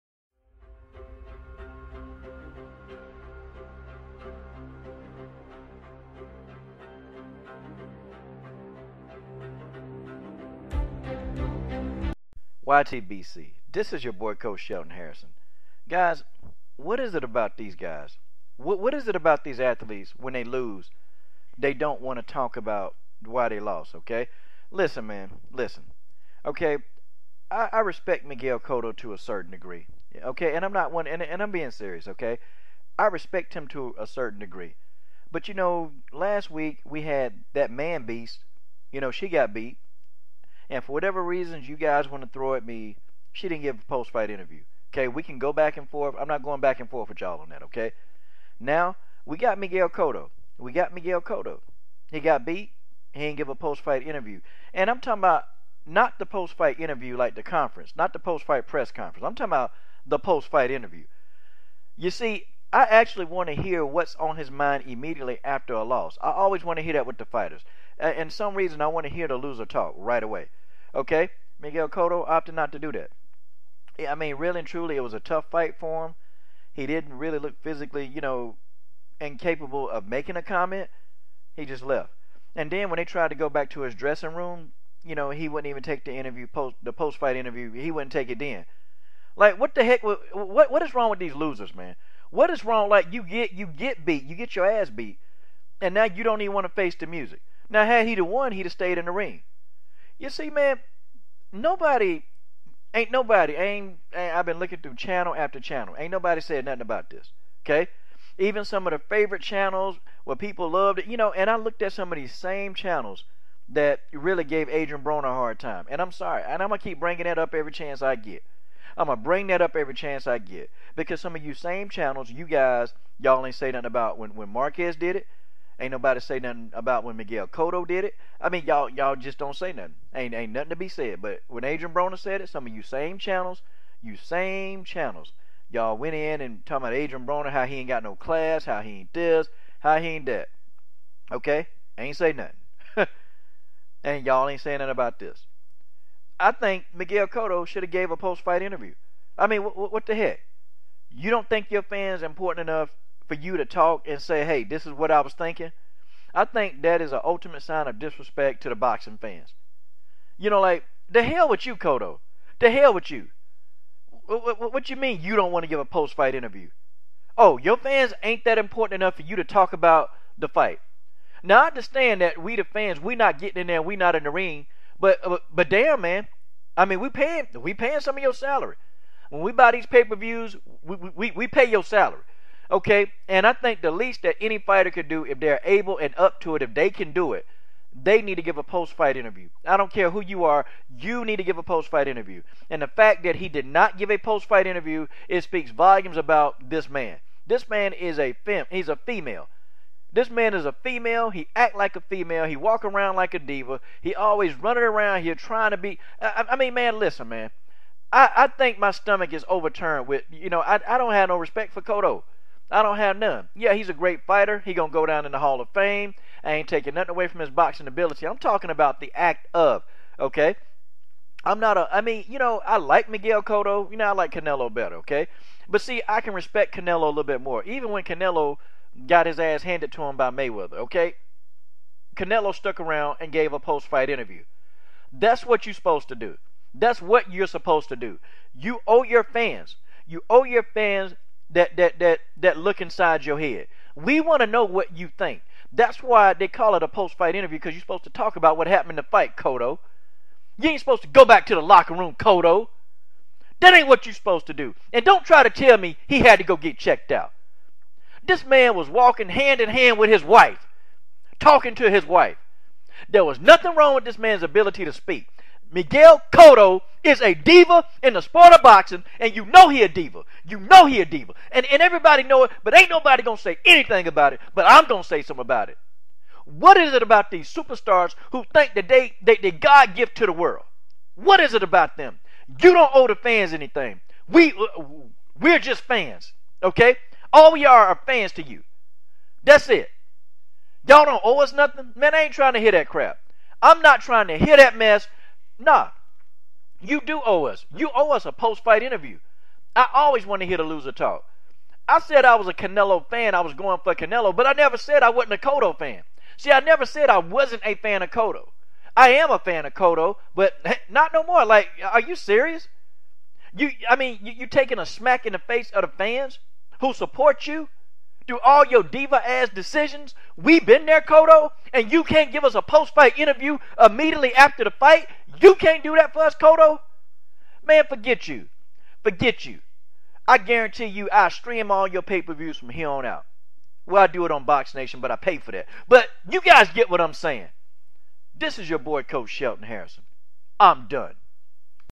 Miguel Cotto No Post Fight Interview Immediately After Fight? Are You Kidding Me??
So Cotto doesn't give a post fight interview? What gives? Coach goes in.